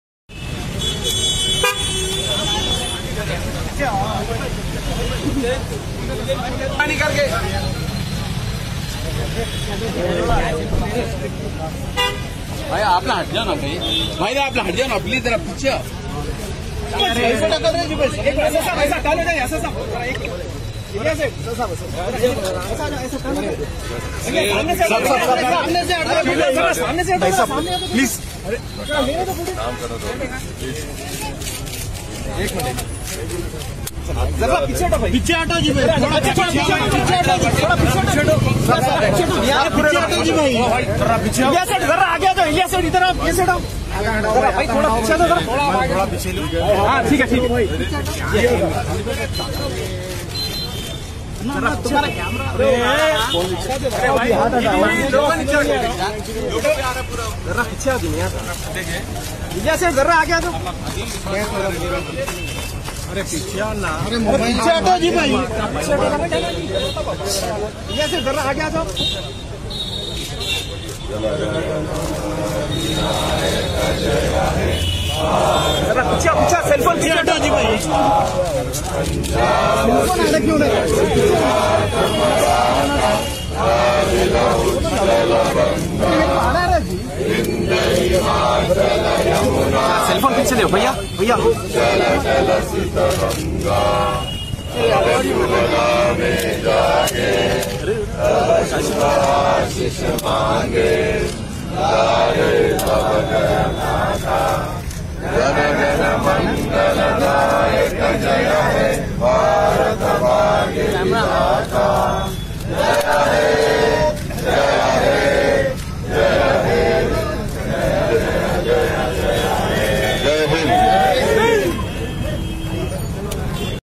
HYDERABAD: AIMIM CHIEF ASADUDDIN OWAISI NEAR MADINA CHOWK INDEPENDENCE DAY...NEWS14
NEWS14...HYDERABAD: AIMIM CHIEF ASADUDDIN OWAISI NEAR MADINA CHOWK INDEPENDENCE DAY...

(NEWS14 ...NAYYER)